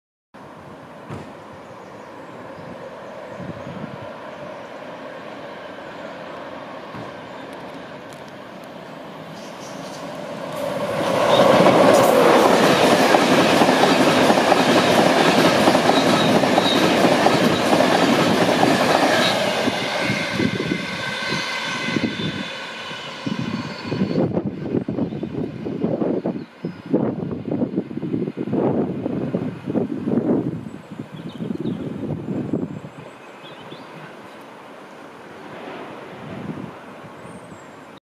Virgin Trains 225 speeds through Durham (14/11/16)
Virgin Trains East Coast Class 91 91107 'Skyfall' hauls 1S09 London King's Cross to Edinburgh through Durham at speed.

14th November 2016.